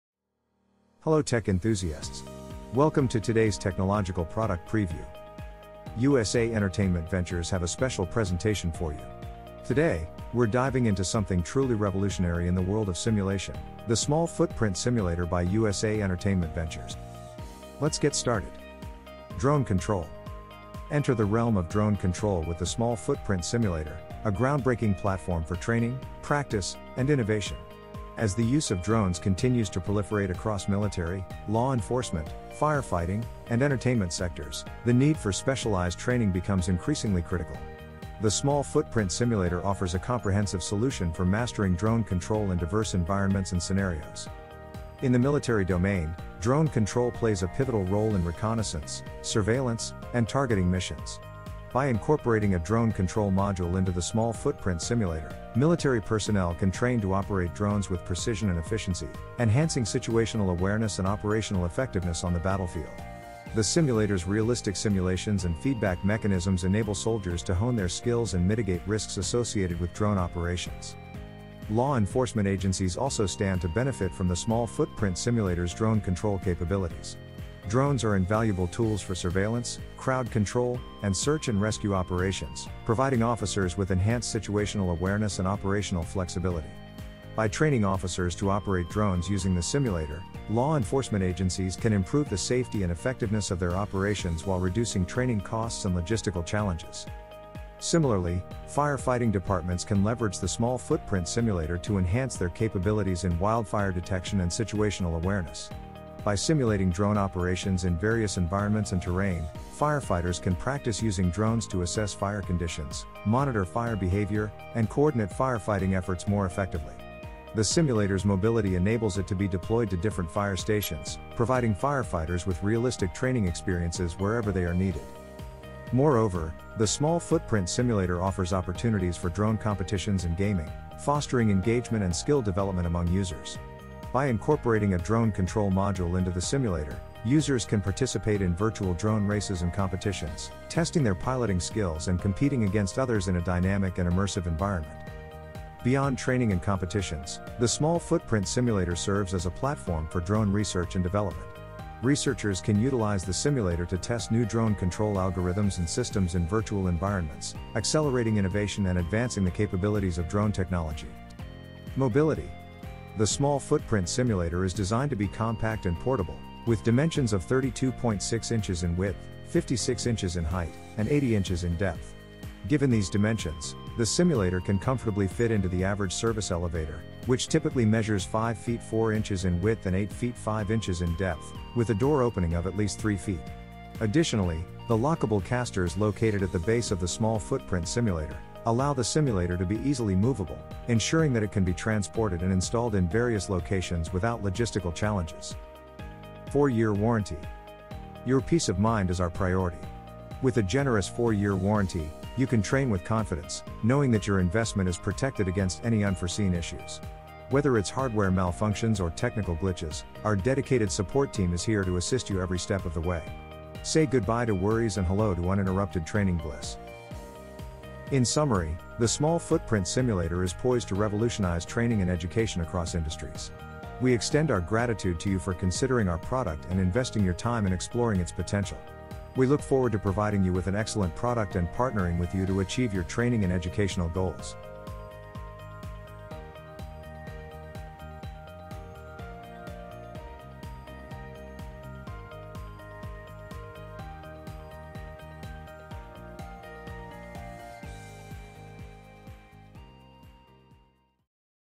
Enhance Drone Operations with the Small Footprint Simulator
The Small Footprint Simulator offers a wide range of benefits for drone control, including training, practice, competitions, research, and development. By providing a realistic and immersive platform for mastering drone operations, the simulator empowers users to enhance their skills, improve operational effectiveness, and drive innovation in drone technology across various fields and industries.

Chapters:

0:00 - Introduction
0:19 - Drone Control
2:41 - Mobility
3:16 - 4-Year Warranty
3:37 - Conclusion